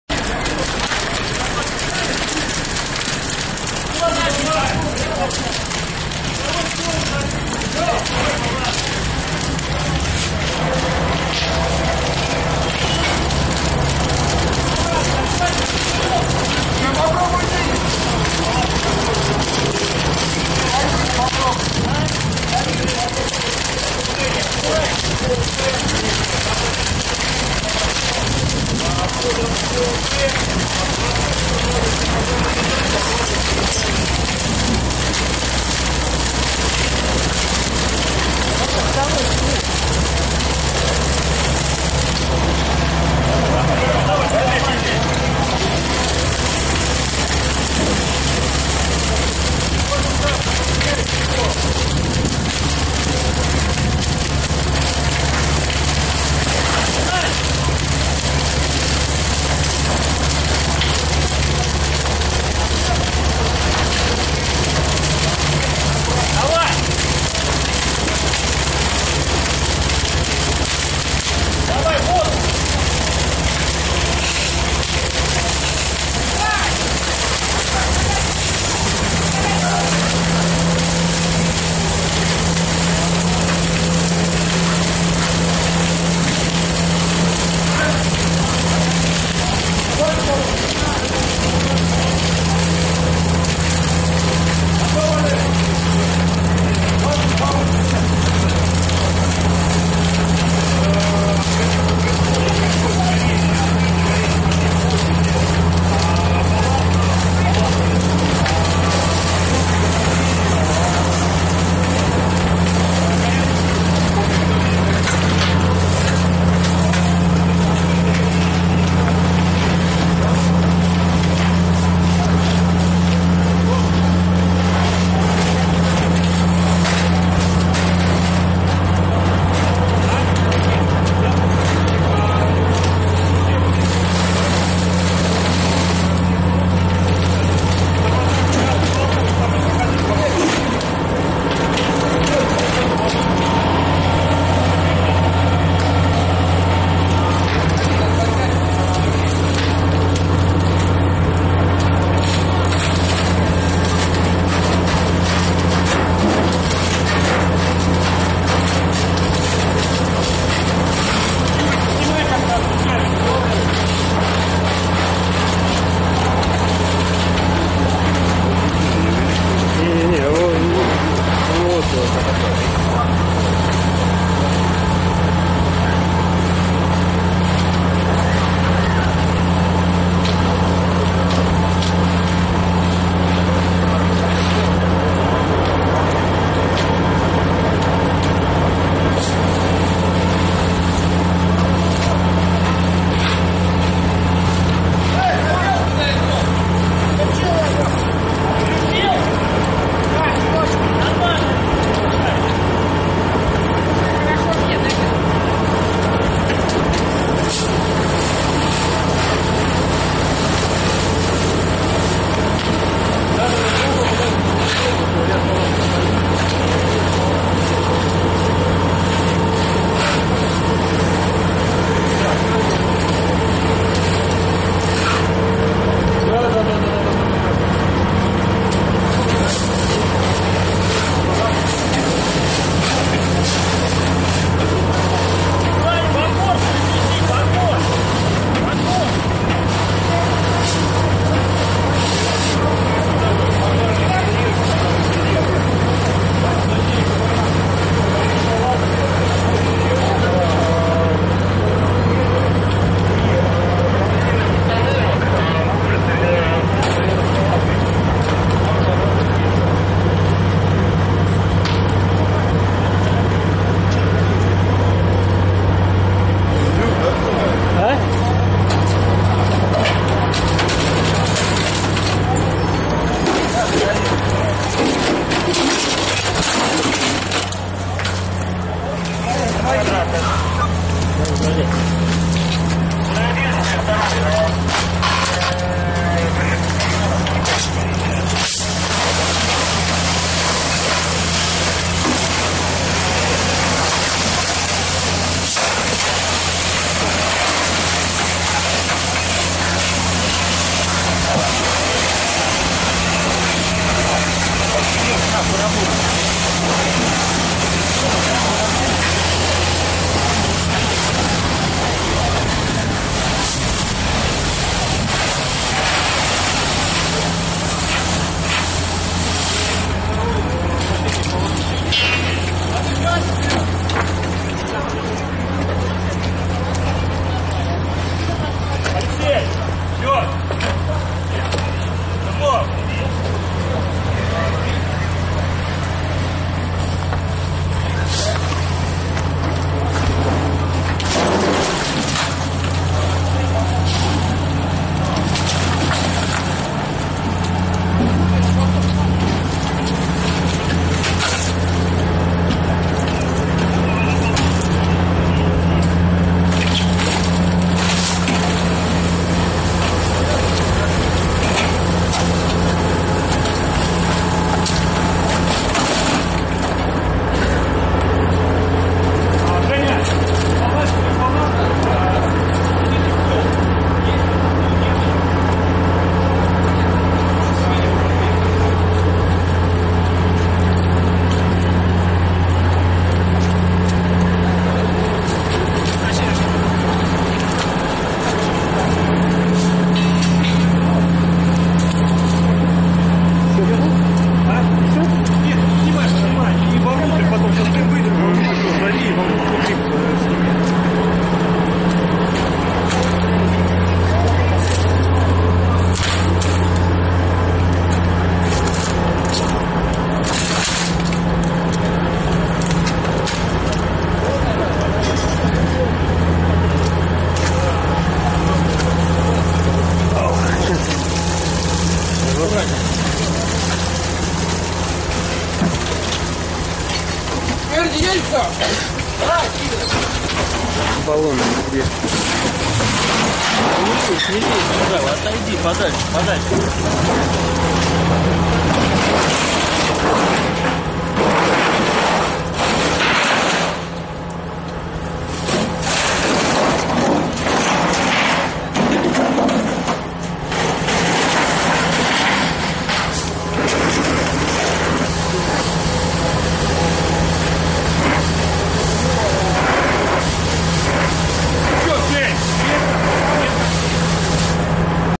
Горит чебуречный ларек
18 марта 2011 года, Зеленоград, 6-й микрорайон. Тушение чебуречного ларька